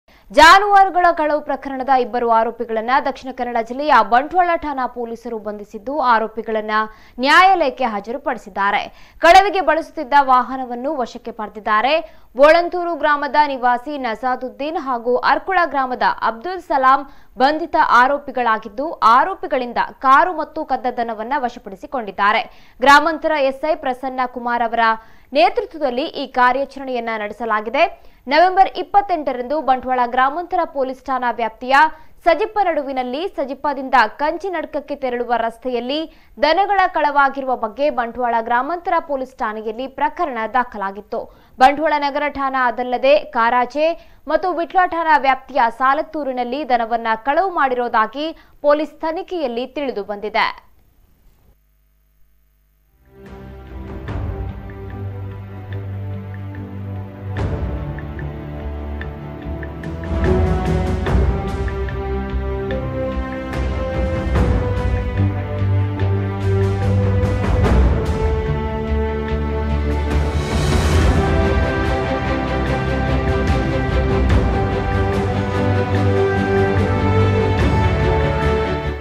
ಬಂಟ್ವಾಳದಲ್ಲಿ ಪೊಲೀಸರ ಬಲೆಗೆ ಬಿದ್ದ ಗೋಕಳ್ಳರು..!
ಜಾನುವಾರುಗಳ ಕಳವು ಪ್ರಕರಣದ ಇಬ್ಬರು ಆರೋಪಿಗಳನ್ನು ದಕ್ಷಿಣ ಕನ್ನಡ ಜಿಲ್ಲೆಯ ಬಂಟ್ವಾಳ  ಠಾಣಾ ಪೊಲೀಸರು ಬಂಧಿಸಿದ್ದು ಆರೋಪಿಗಳನ್ನು ನ್ಯಾಯಾಲಯಕ್ಕೆ ಹಾಜರುಪಡಿಸಿದ್ದಾರೆ. ಕಳವಿಗೆ ಬಳಸುತ್ತಿದ್ದ ವಾಹನವನ್ನೂ ವಶಕ್ಕೆ ಪಡೆದಿದ್ದಾರೆ. ಬೋಳಂತೂರು ಗ್ರಾಮದ ನಿವಾಸಿ ನಜಾದುದ್ದೀನ್ ಹಾಗೂ ಅರ್ಕುಳ ಗ್ರಾಮದ ಅಬ್ದುಲ್ ಸಲಾಂ ಬಂಧಿತ ಆರೋಪಿಗಳಾಗಿದ್ದು, ಆರೋಪಿಗಳಿಂದ ಕಾರು ಮತ್ತು ಕದ್ದ ದನವನ್ನು ವಶಪಡಿಸಿಕೊಂಡಿದ್ದಾರೆ.ಗ್ರಾಮಾಂತರ ಎಸ್ಸೈ ಪ್ರಸನ್ನ ಕುಮಾರ್ ಅವರ ನೇತೃತ್ವದಲ್ಲಿ ಈ ಕಾರ್ಯಾಚರಣೆ ನಡೆಸಲಾಗಿದೆ.